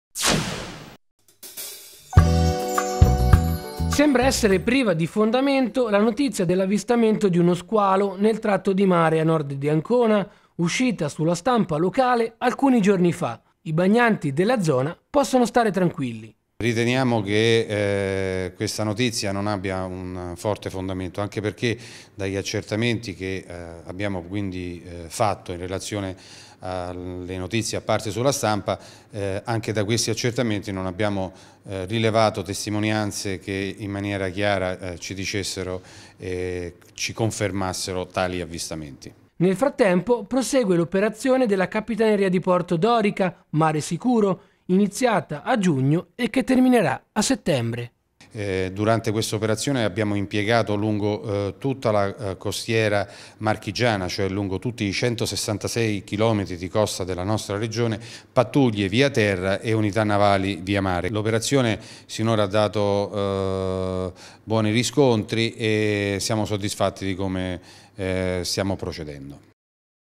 CAPITANERIA DI PORTO ANCONA, PROSEGUE L'OPERAZIONE "MARE SICURO" - 7x4 TV
Sembra essere priva di fondamento la notizia dell'avvistamento di uno squalo nel tratto di mare a nord di Ancona  uscita sulla stampa locale alcuni giorni fa. I bagnanti della zona possono stare tranquilli. Nel frattempo prosegue l'operazione della Capitaneria di porto dorica "Mare sicuro 2011" iniziata a giugno e che terminerà a settembre.